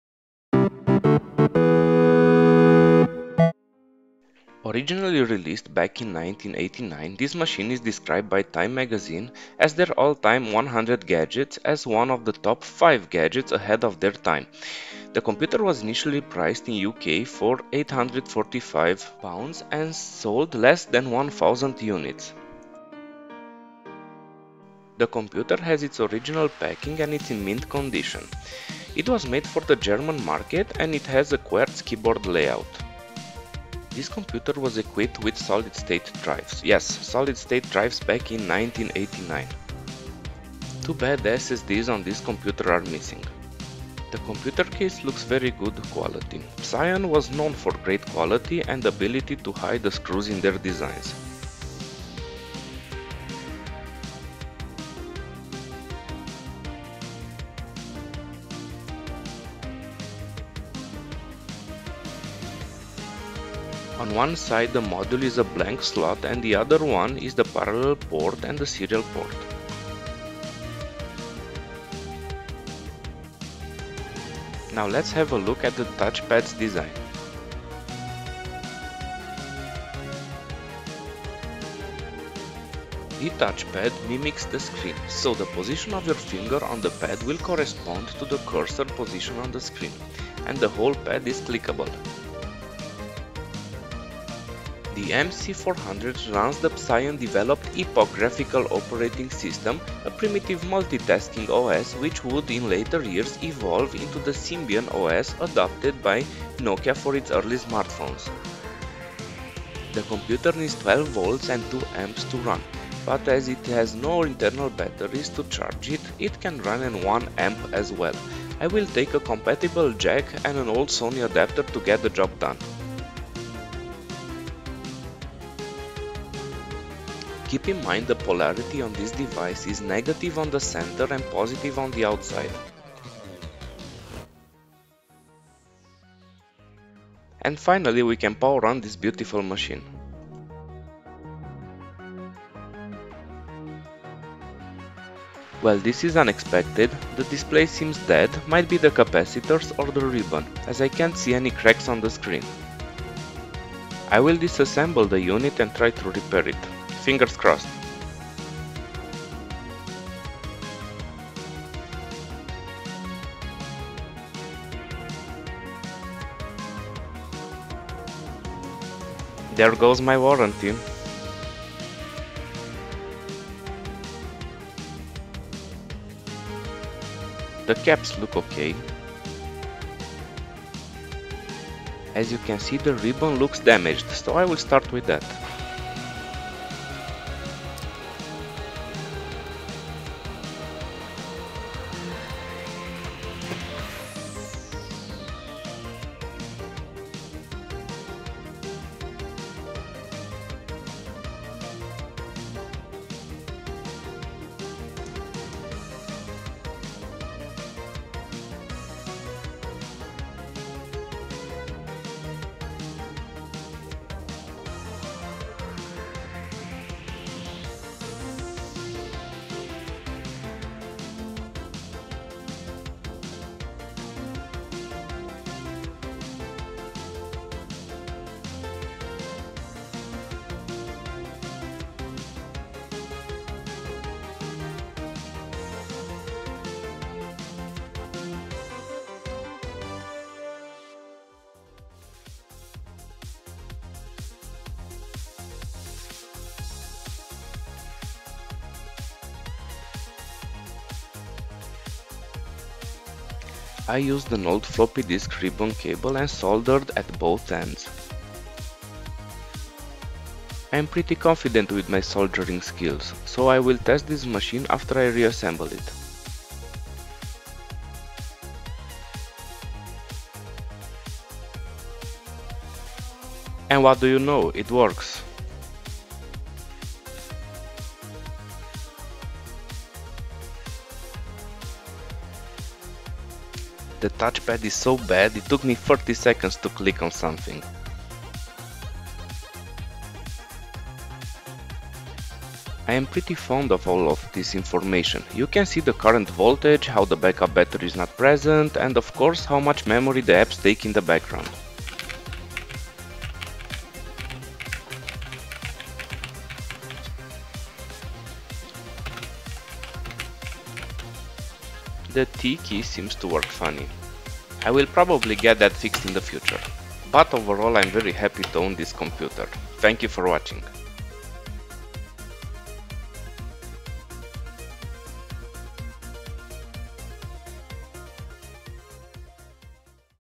PSION Mobile Computer MC 400 (overview, tear down, repairs)
Overview, tear down and repairs done on my new old stock, PSION Mobile Computer MC400.
The first commercially successful touch-pad was developed for Psion‘s MC 200/400/600/WORD Series in 1989.